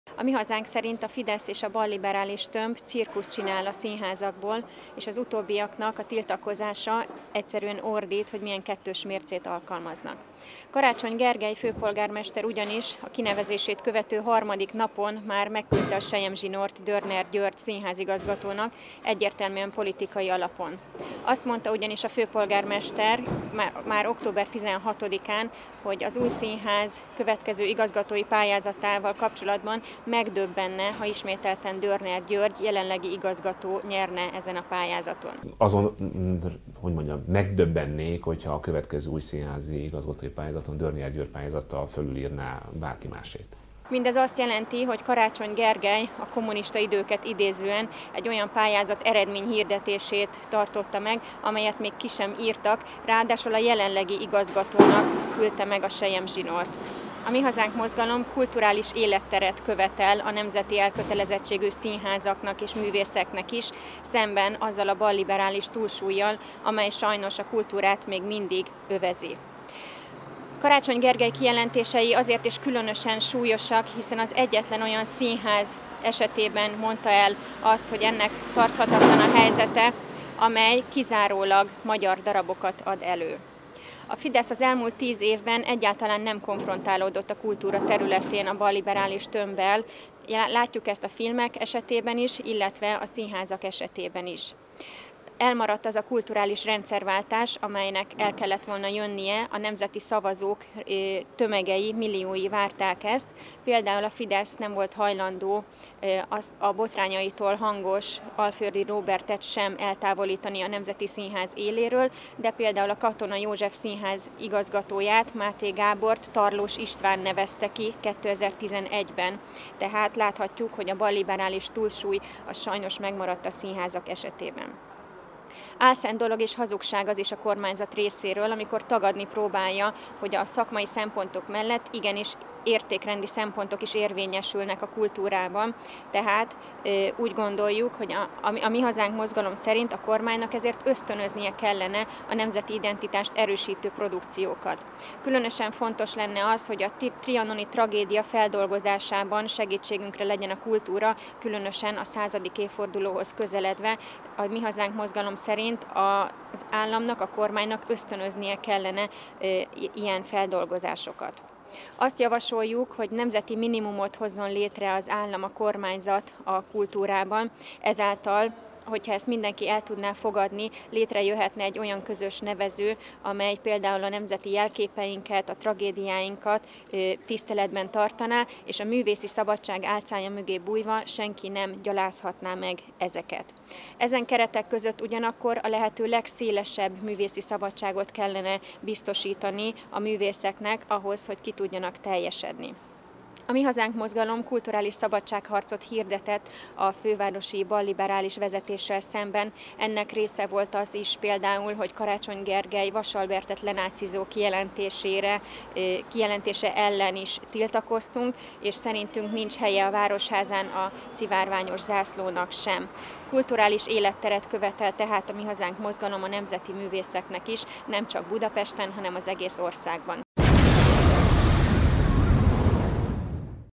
El a kezekkel az Újszínháztól!
A Mi Hazánk kulturális szabadságharcot hirdetett a fővárosi balliberális vezetéssel szemben. Most az Újszínház vezetője, Dörner György mellett állunk ki, akinek Karácsony főpolgármester selyemzsinórt küldött. Mindez három nappal az önkormányzati választás után történt.